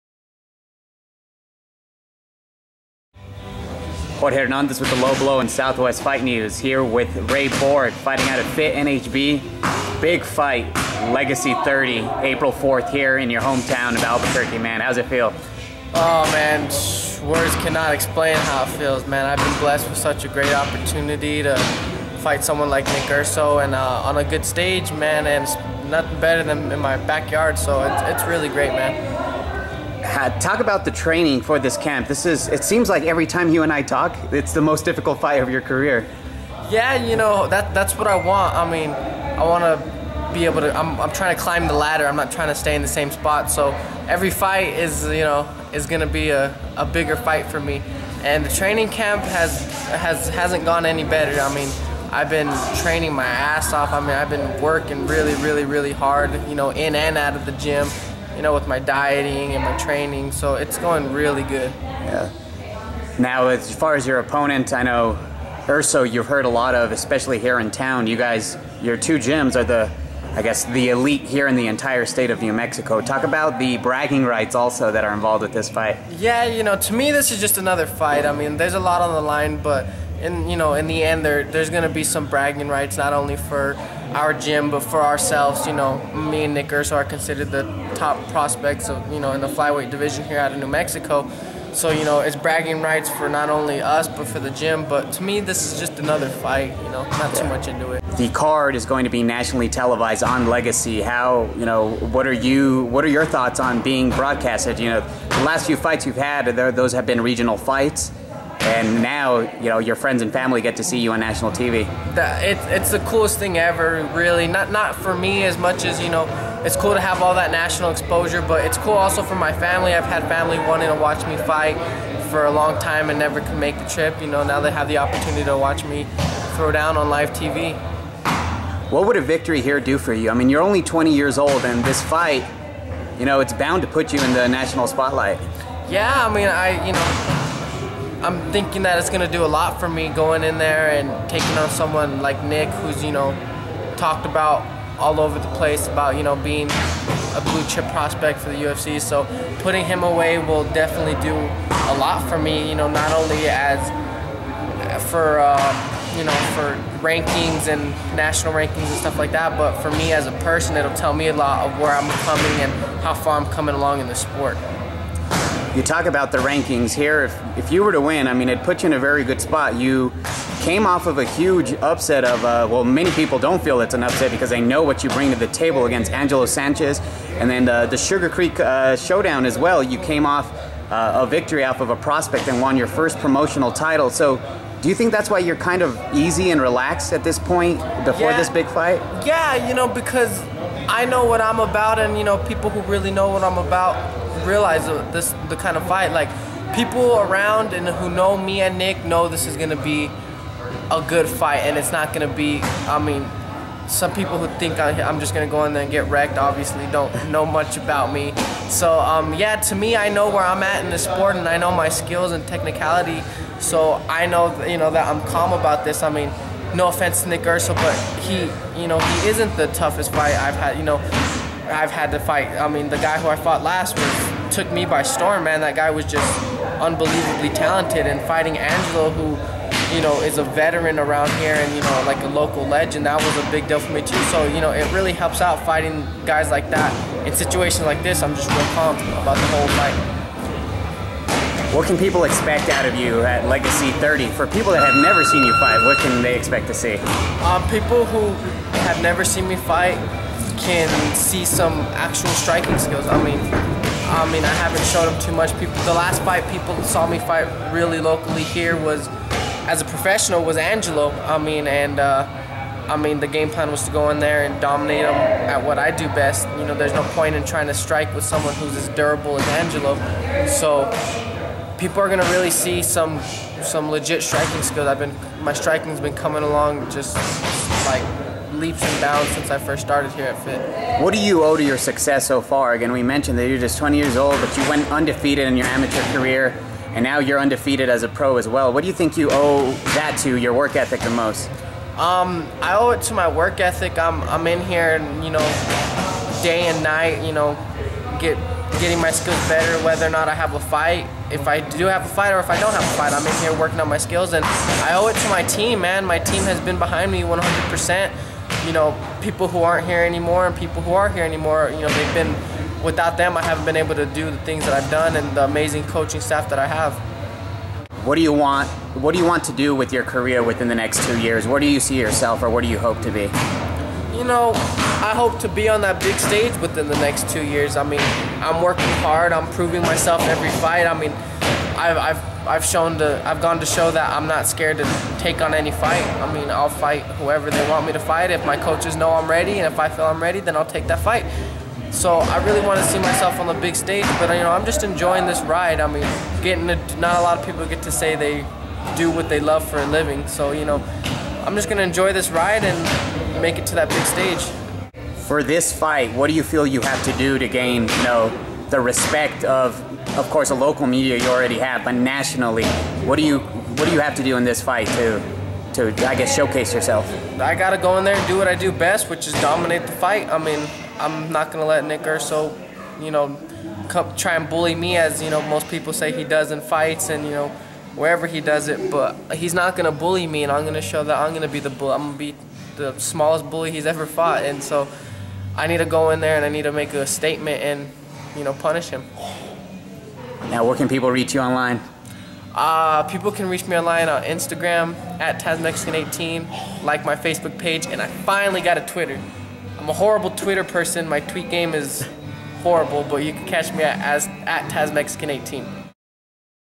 Ray Borg vs. Nick Urso Flyweight Division and Gym Bragging Rights Up for Grabs
Undefeated flyweight Ray Borg says that despite his Legacy FC 30 match vs. Nick Urso being for New Mexico flyweight and Albuquerque gym bragging rights, it's just another fight. Many feel that despite Holly Holm and Juliana Werner being main event, Borg vs. Urso will be fight of the night.